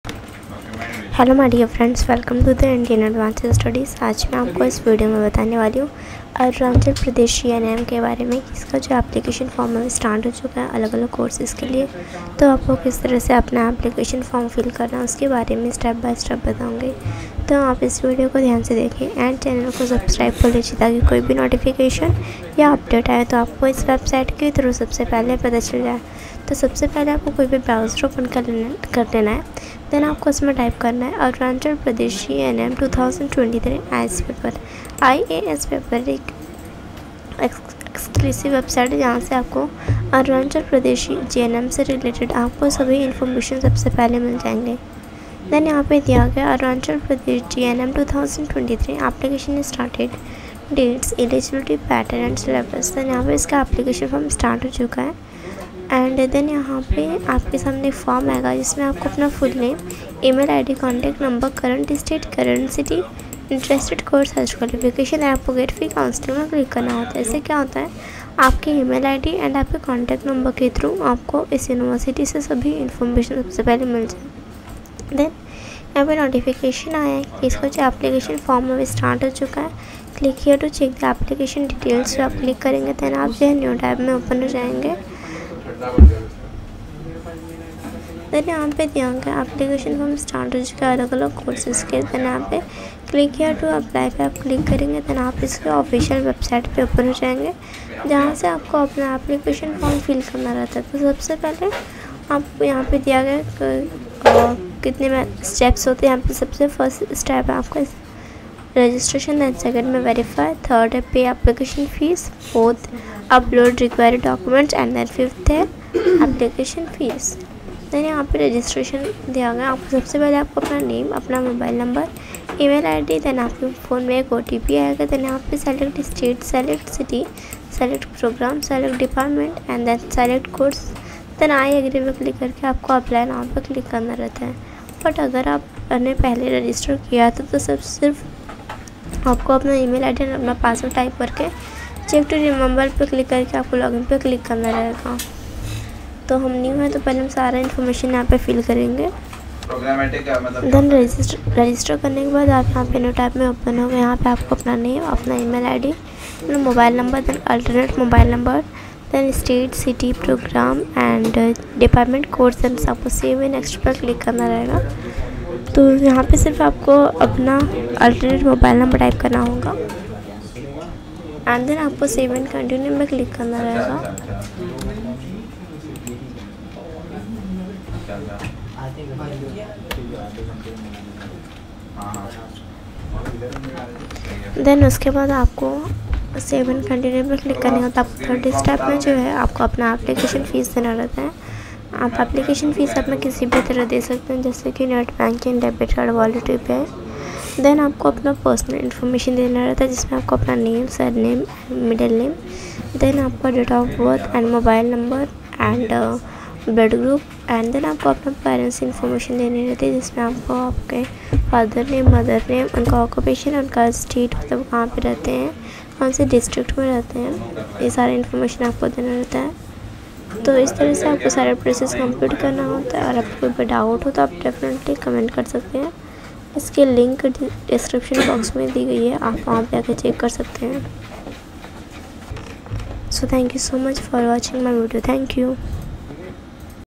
Arunachal Pradesh GNM 2023 Application Form (Out)- How to Fill Application Form Online Mode
Arunachal Pradesh GNM 2023 is conducted by Arunachal University. The Full Form of the GNM course is known by General Nursing and Midwifery. It is a 3½ years diploma in paramedics. Nursing and Midwifery aim to the establishment of care for individuals, families, and communities.